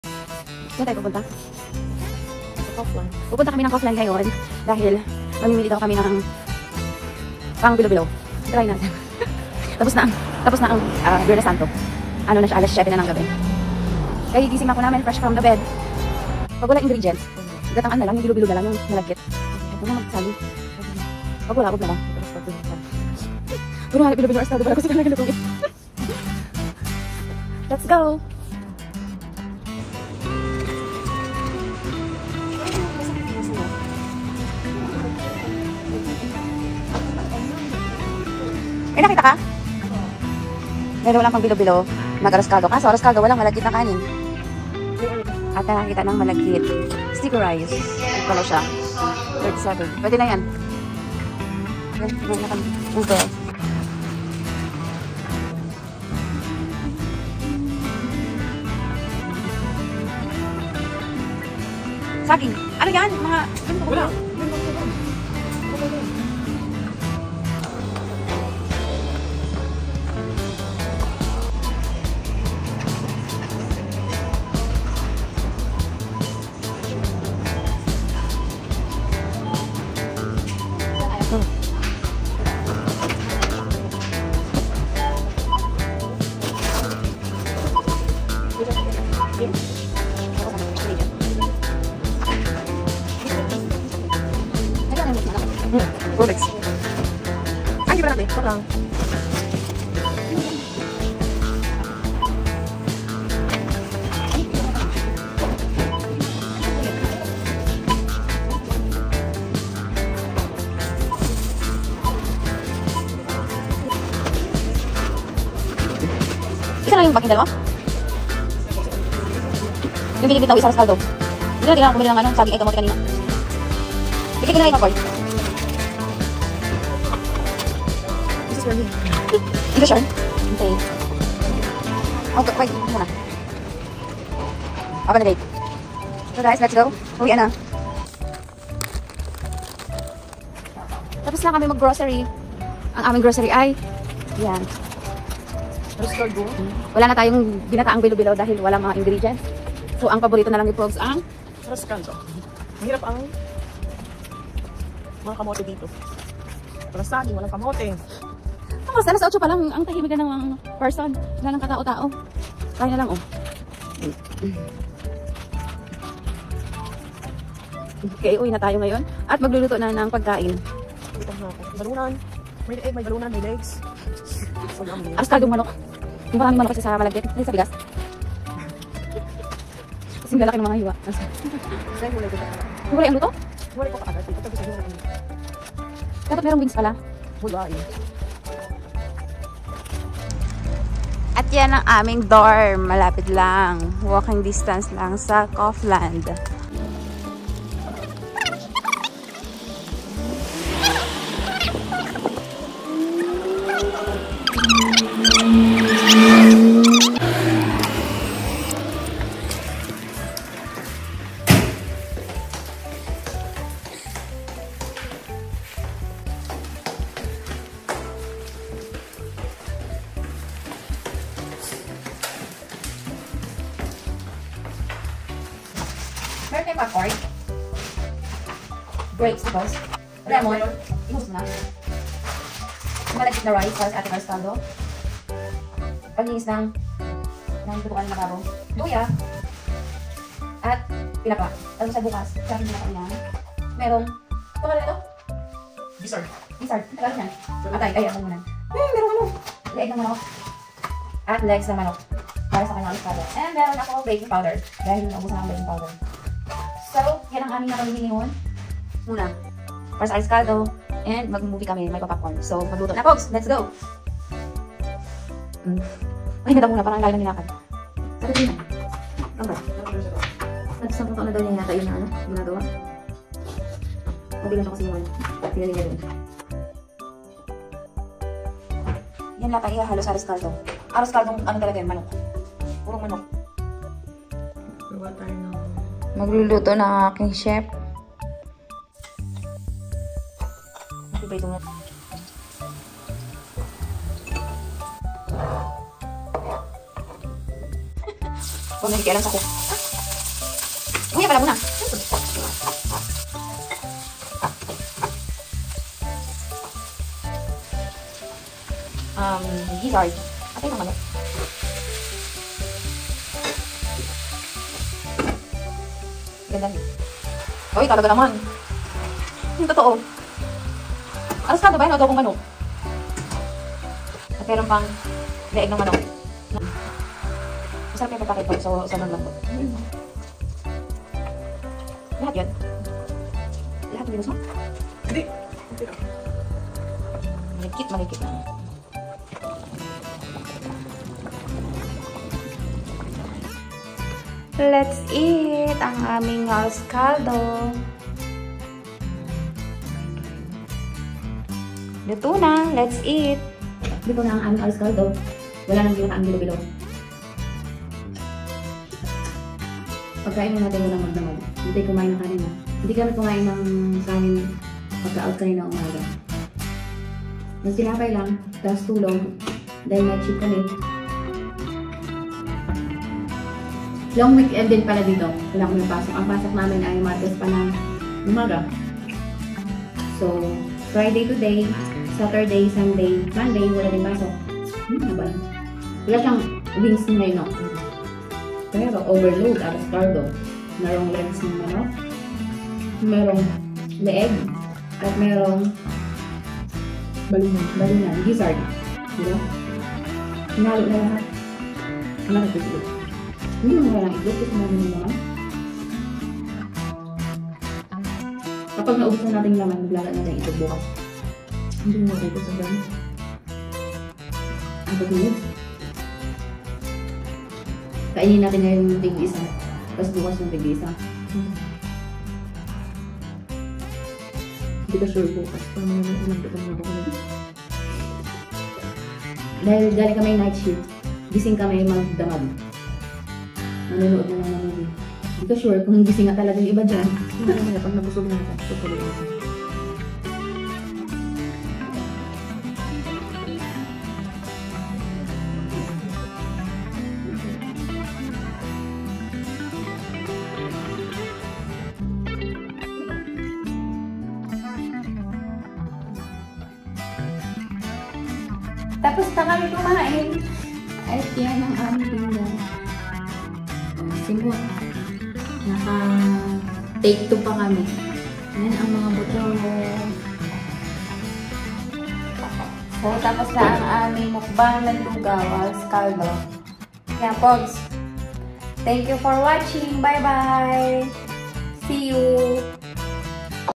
NAG GROCERY PANG ARROZ CALDO| LONG WEEKEND| ANALYN DG
Nag grocery kame sa Kaufland at nag luto ng Arroz Caldo para sa Mahal na araw. Long weekend din ang DHL Czech Republic. Holiday season kaya sulitin ang ang araw sa pahinga at food trip.